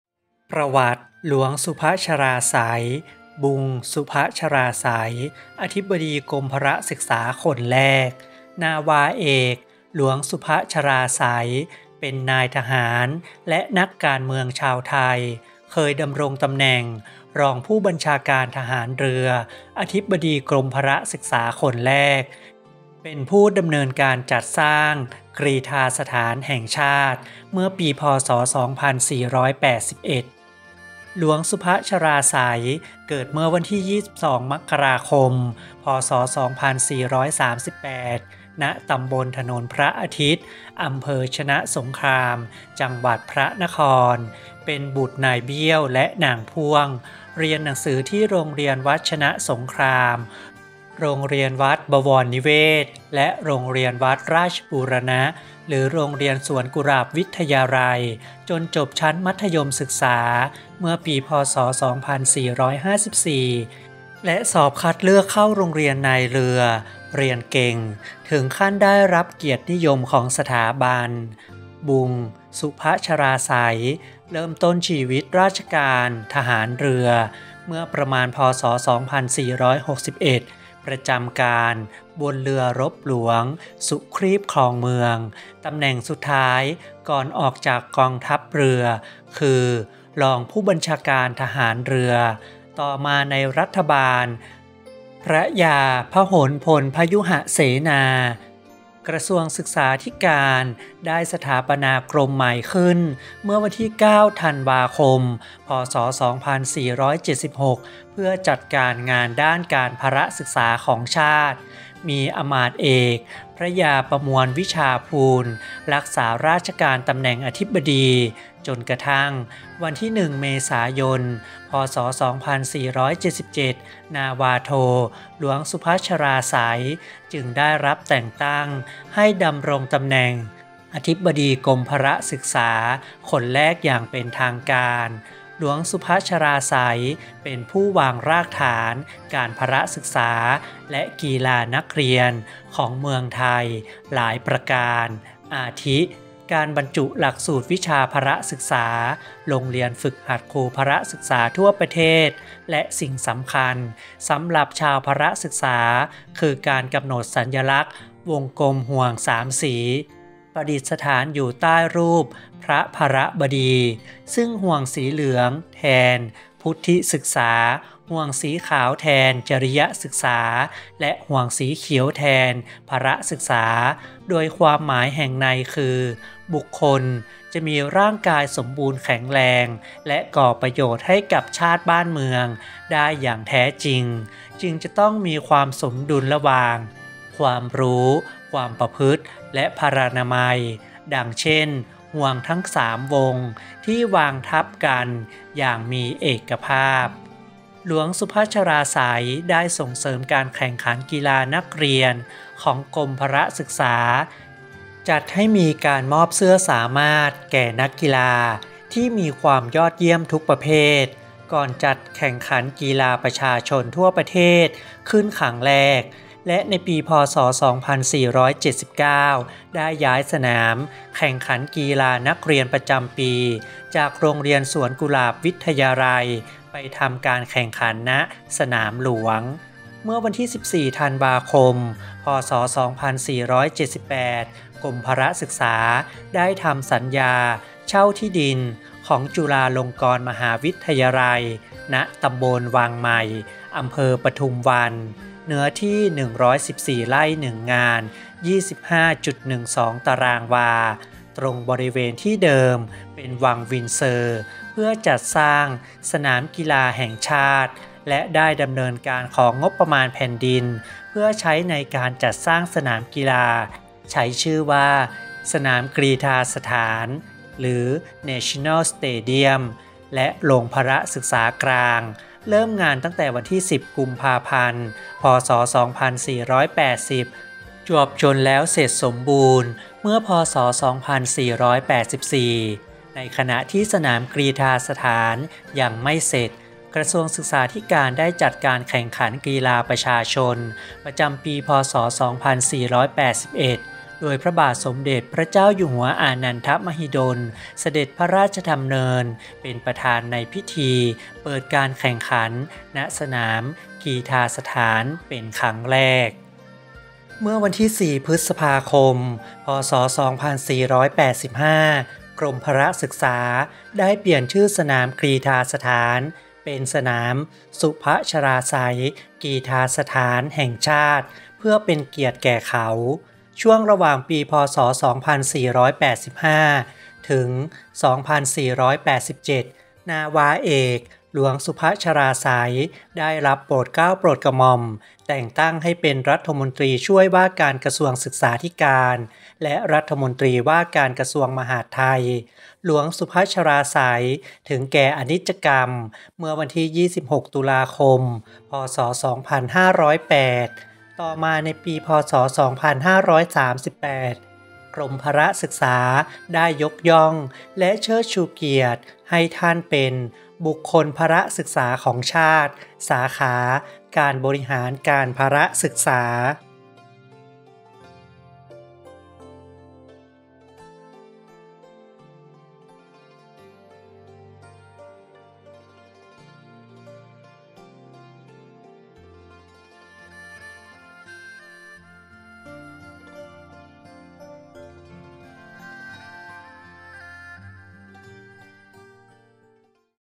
หลวงศุภชลาศัย
นาวาเอก หลวงศุภชลาศัย (บุง ศุภชลาศัย) เป็นนายทหารและนักการเมืองชาวไทย เคยดำรงตำแหน่งรองผู้บัญชาการทหารเรือ อธิบดีกรมพลศึกษาคนแรก เป็นผู้ดำเนินการจัดสร้าง กรีฑาสถานแห่งชาติ เมื่อ พ.ศ. 2481